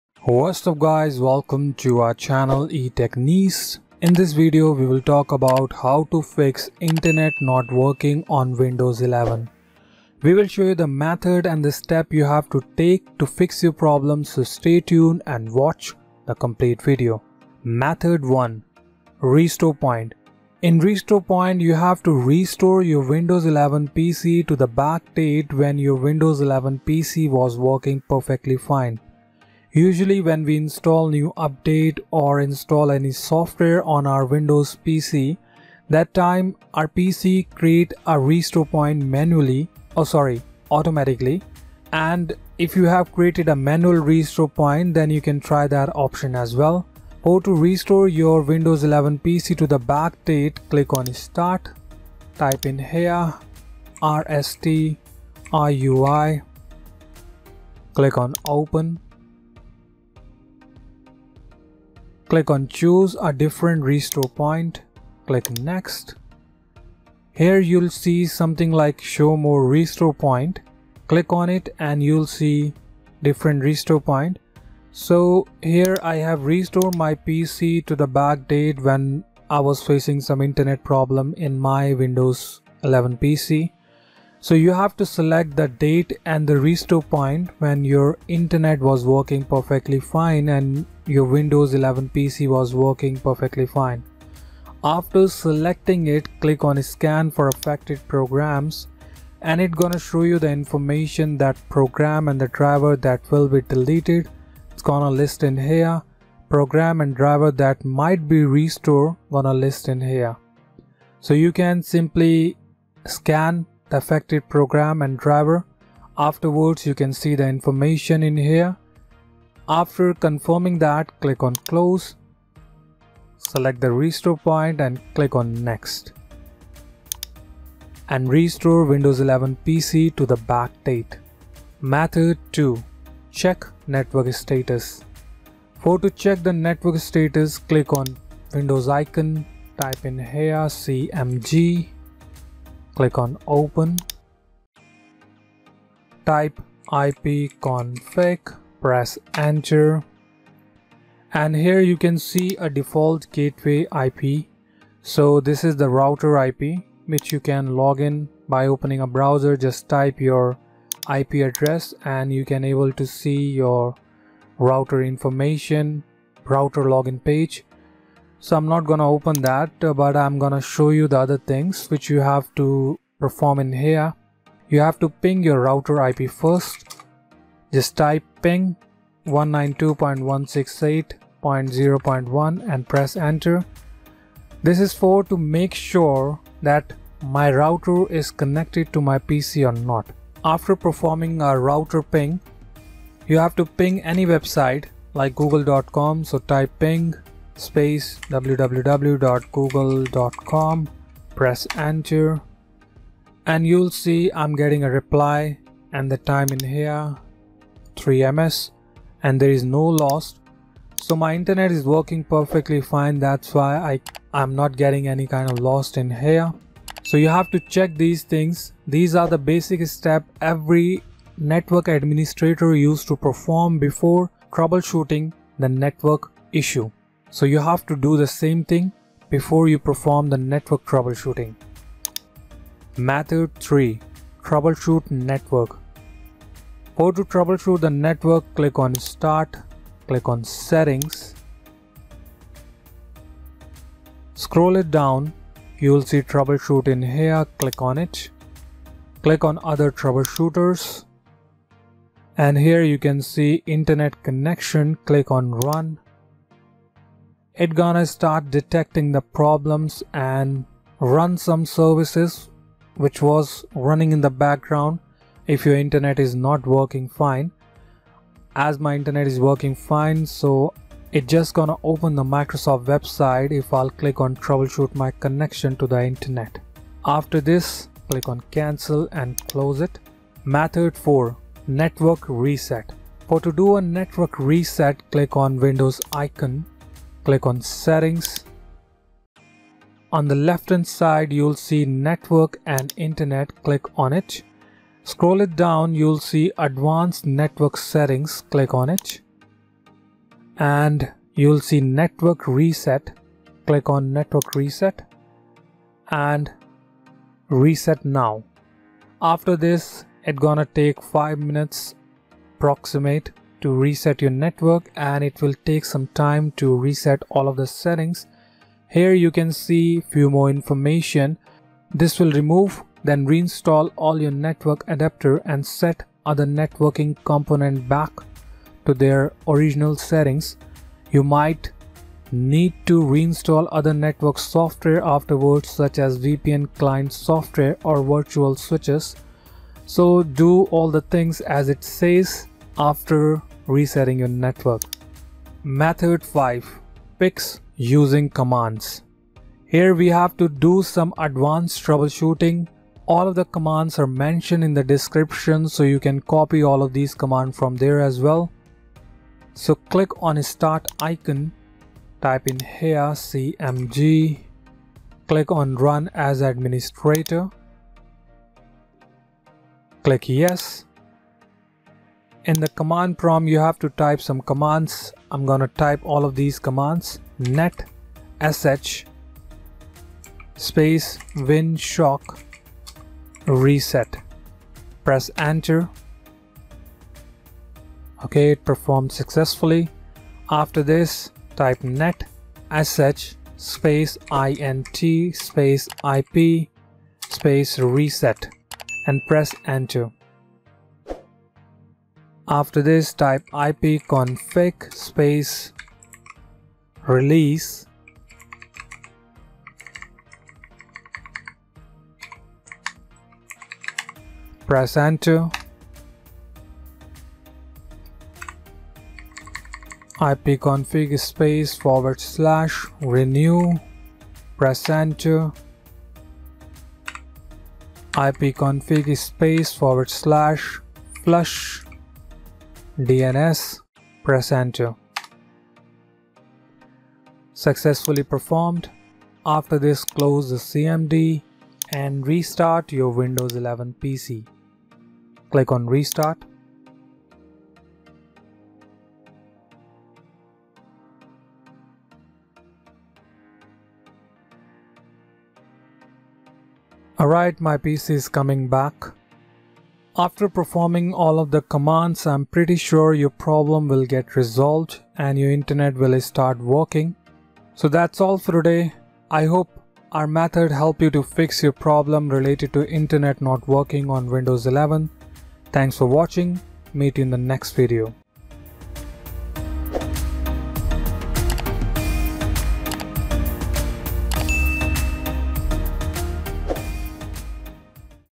How to Fix Internet Connection Problem Windows 11 ( 2025 )
We explained how to fix internet connection problem windows 11 & methods to fix network reset command/network reset cmd windows 11 / network reset dell laptop.

Topic Addressed in Video :

network reset error windows 11 laptop no wifi option
laptop no wifi adapter
laptop no wifi icon